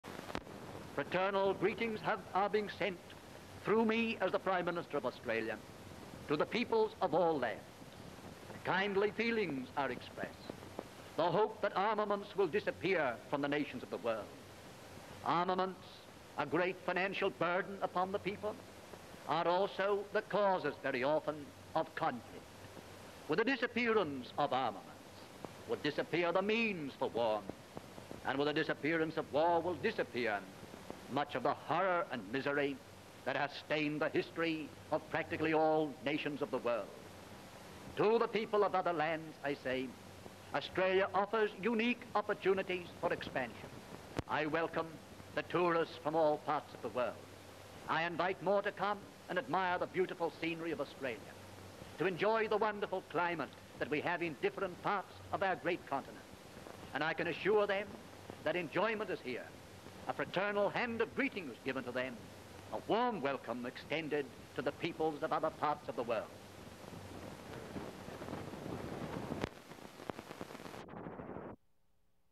PM James Scullin on disarmament and peace (1929)
Prime Minister of Australia, James Henry Scullin, speaks about conflict and how it could end through disarmament, and invites people to travel to the Australian continent.

Title Scullin on disarmament--outtakes
Date Note Filmed in 1929.
Location     Australia
Newsreel Producer Fox Movietone News (Production unit)
Physical Format Film: 35mm. Sound. B&W. Negative. 1.15:1 (Movietone). Variable-density optical. Nitrate.
Citation Scullin on disarmament--outtakes. (Fox Movietone News Story 4-444.) Fox Movietone News Collection. Moving Image Research Collections. University of South Carolina.